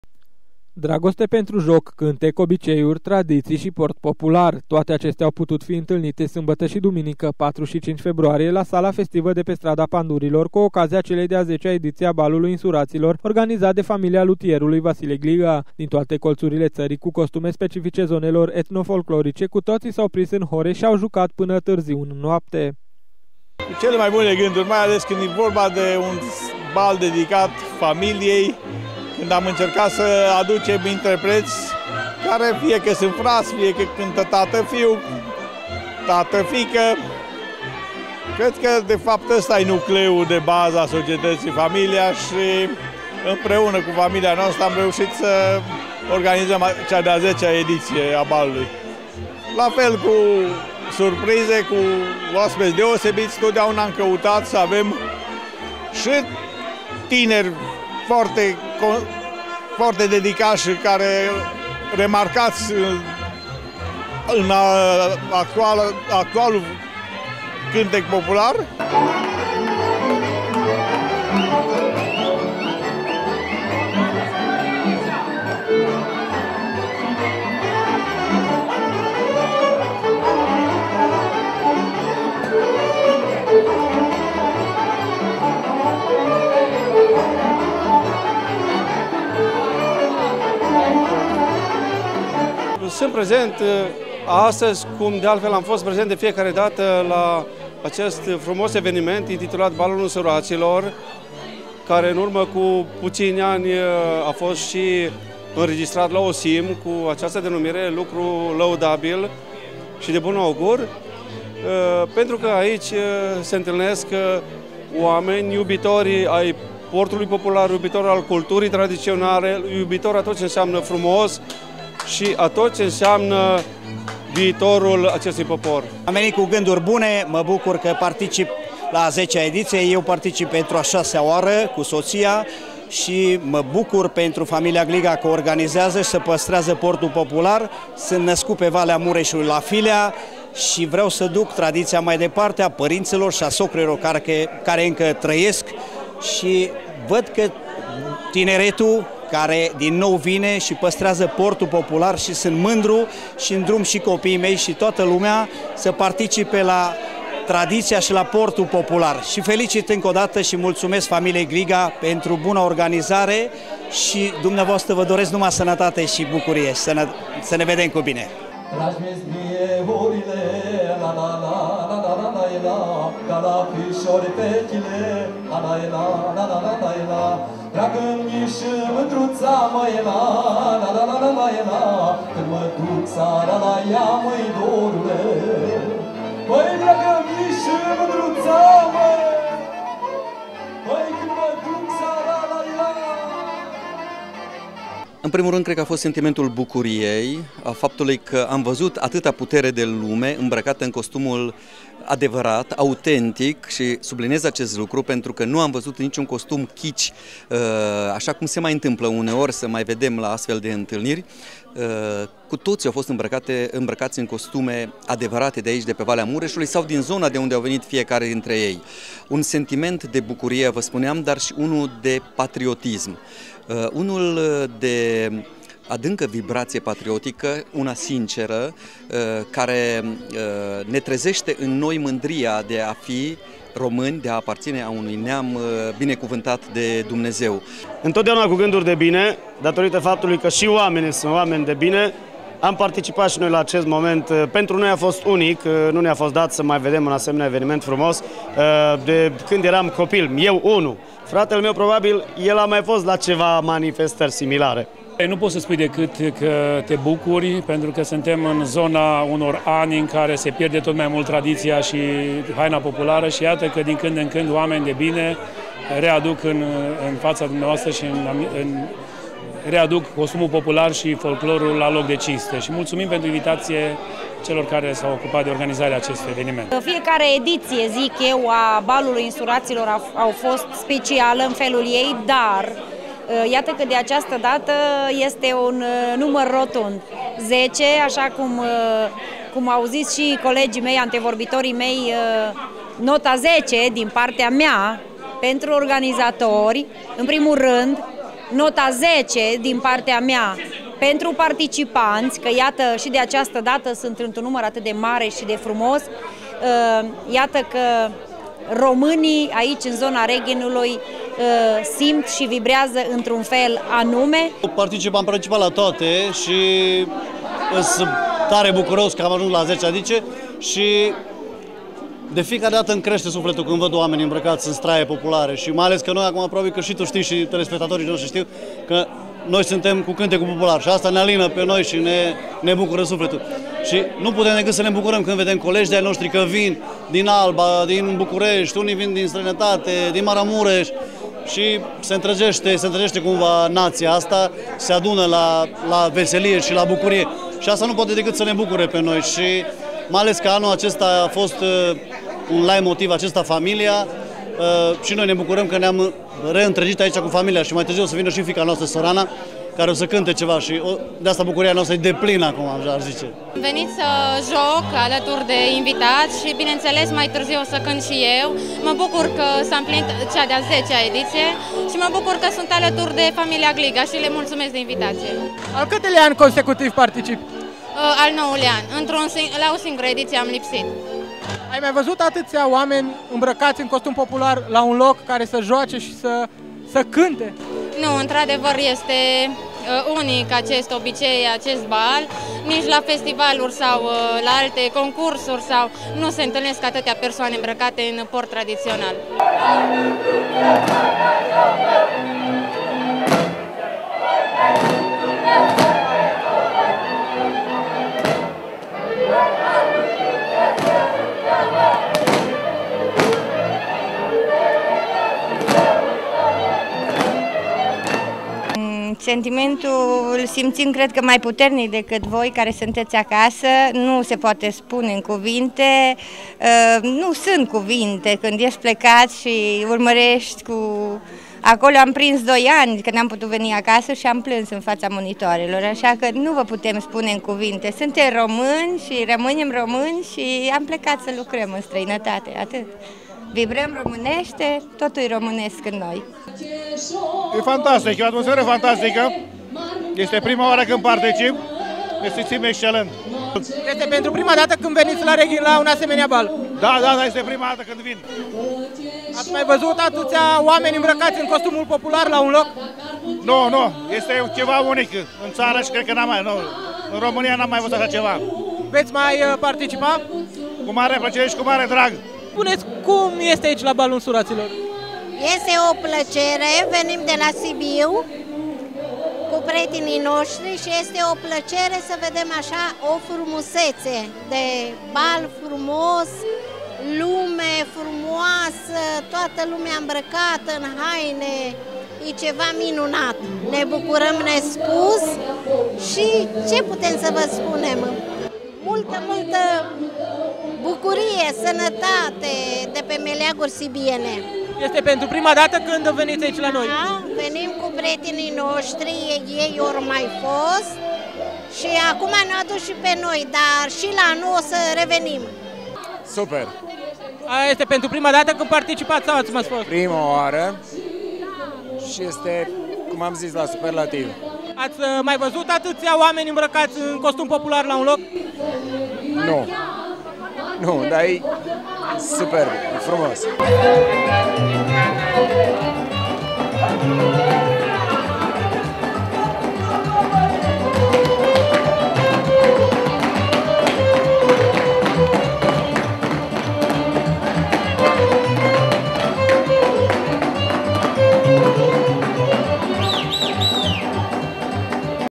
_”BALUL ÎNSURAŢILOR” A ÎMPLINIT 10 ANI (2017 02 06)
Cea de-a 10-a ediţie a „Balului Însuraţilor” organizat de familia lutierului Vasile Gliga a adunat în 4 respectiv 5 februarie sute de persoane care iubesc folclorul şi care preţuiesc portul popular. Oaspeţi din toate colţurile ţării dar şi din străinatate au răspuns prezent celui mai mare eveniment de acest gen din ţară. Ediţia din acest an a avut şi o temă simbolică, „familia”.